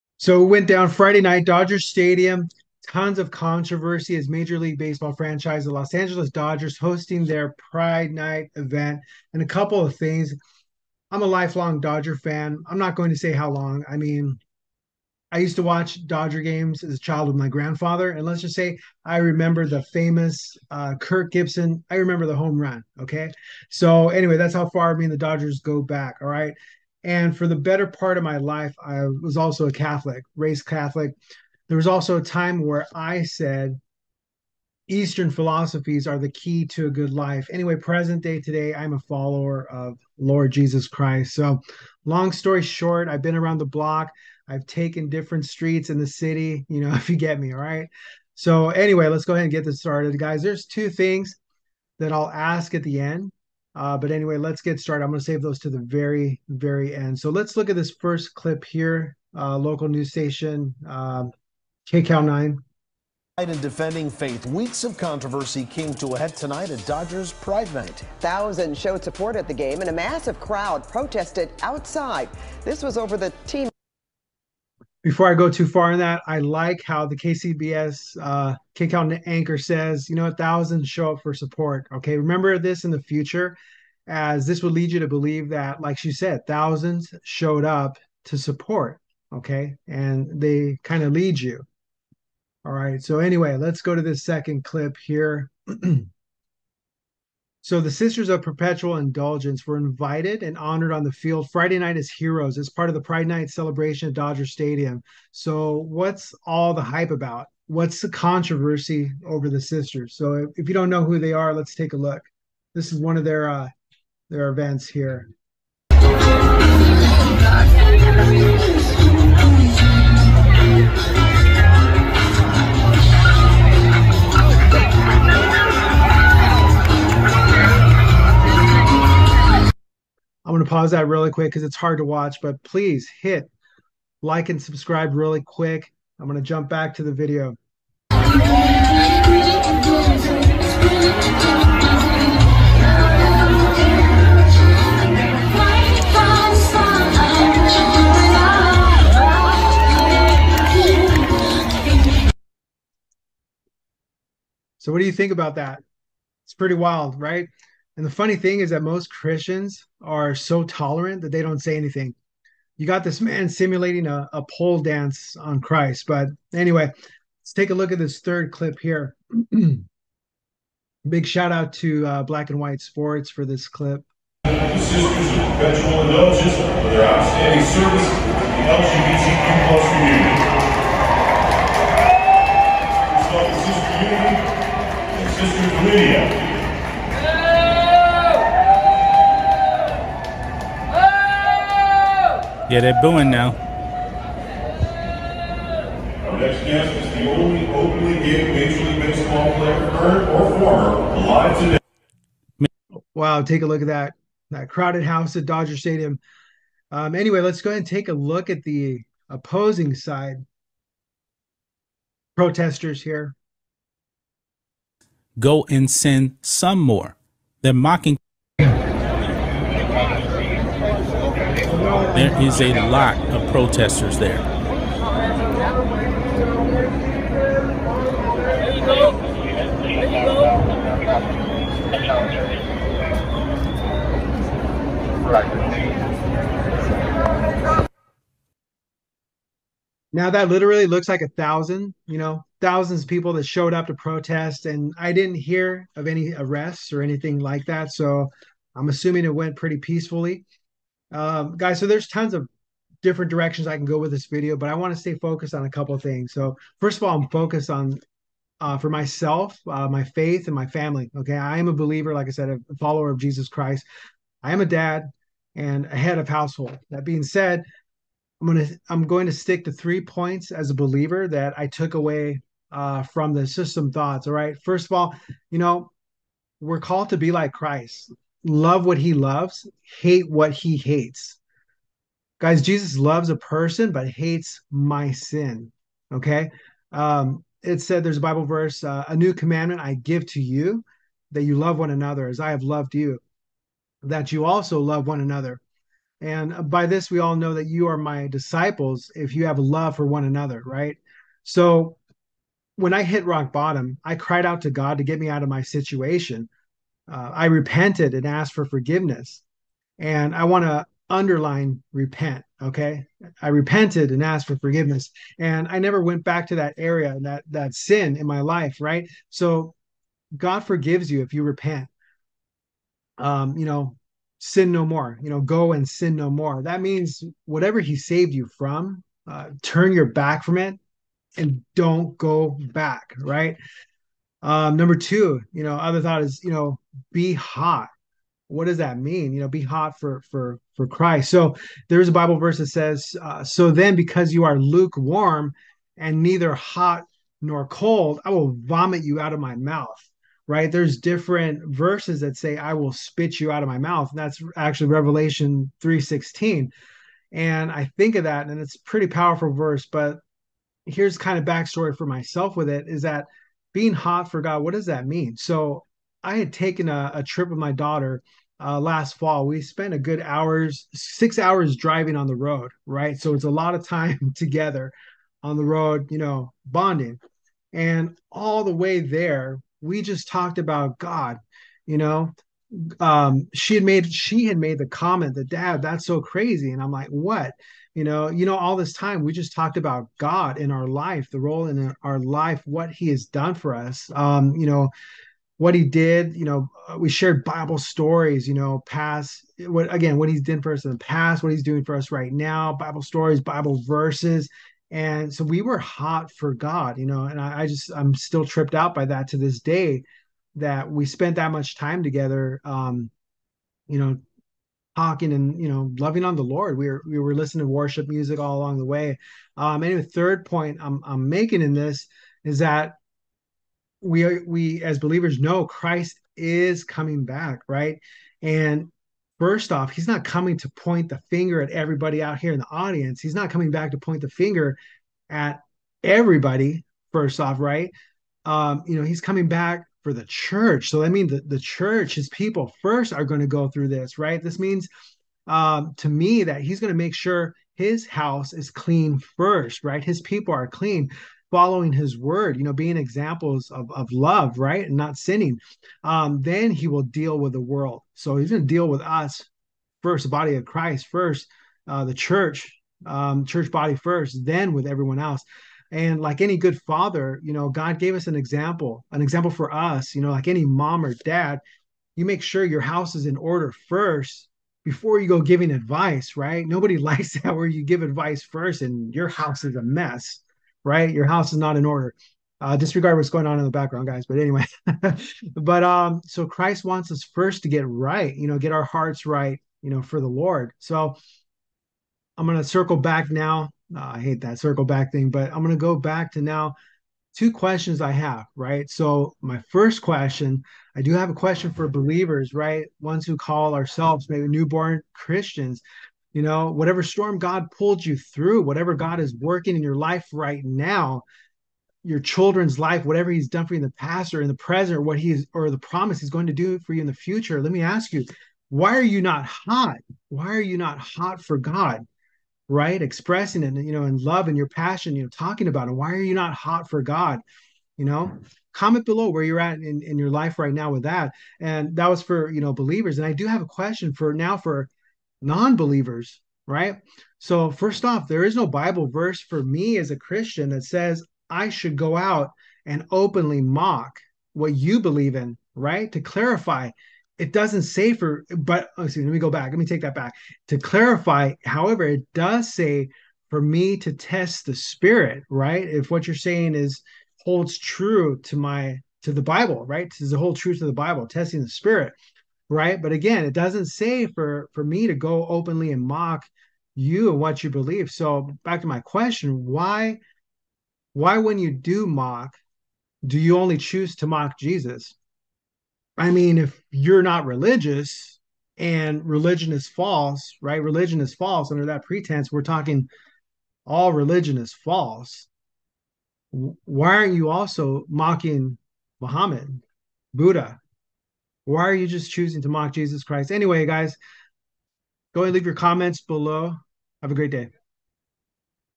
Thousands show up to protest Los Angeles Dodgers Pride Night!
Friday, June 16, 2023, Major League Baseball ⚾️ franchise the Los Angeles Dodgers 🧢 hosted their Pride Night event. Not without controversy …
The Dodgers invited, un-invited, and re-invited Sisters of Perpetual Indulgence.
There’s 3 Takeaways from this for “believer’s we need to be praying about in our own lives. ⬇️
1️⃣ - Be like Jesus, love what he loves ❤️, hate what he hates. For clarification, Jesus does love us. But that’s not fully true … He loves us, and he hates sin. The key many of us miss, is that we ask for forgiveness, and we never repent! He forgave me, and he can forgive you, but we must include repentance and turning away from that sin.
2️⃣ - Be hot 🔥 for God. There’s the bible verse “So then, because you are lukewarm, and neither cold nor hot, I will vomit you out of My mouth.” - Revelation 3:16. What does that mean to you? Is my worship with all my heart, do I speak of God with all my heart, etc.
3️⃣ -  Christ is coming back 🚨. He’s coming back to clean up his church first.  Like any good Father, Mother, or leader, we make sure our house is clean before we go giving advice elsewhere. Christ wants us to get our heart right first. When we don’t repent and change, we don’t have any testimony of what God did for us, and others are not moved when they see our heart is not right and we seem to live 2 different ways.
My 2 questions … You’ll have to watch for those!
I hope this added some value, gave you hope, or at least made you re-think 🤔